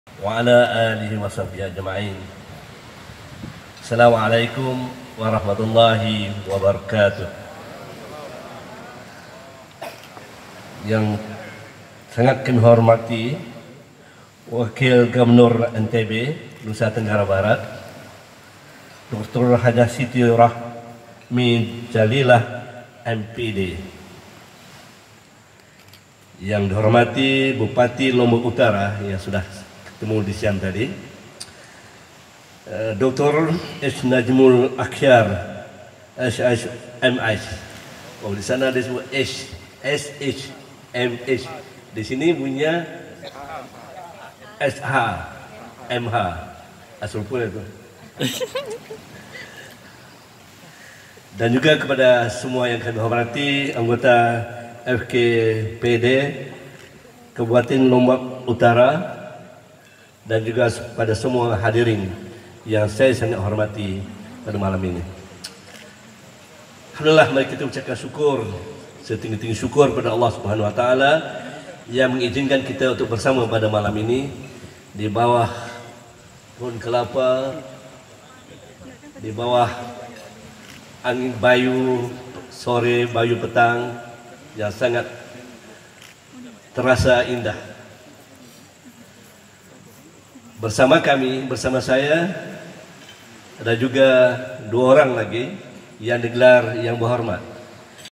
Ucapan Dato' Tengku Hassan mewakili delegasi Kerajaan Negeri Terengganu
Majlis Makan Malam meraikan Delegasi Kerjaan Negeri Terengganu bersempena lawatan ke KLU
1 Maret, 2019
Jeeva Klu Hotel